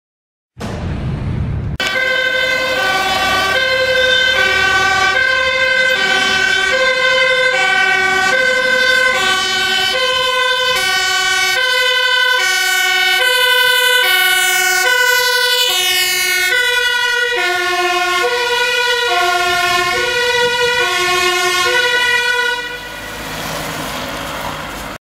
Brandweer en politie met spoed naar een GRIP1 in Drachten
PRIO 1 Woningbrand (GRIP: 1) (NB:Grote Brand) (Ass.: rv) De Lange West  Drachten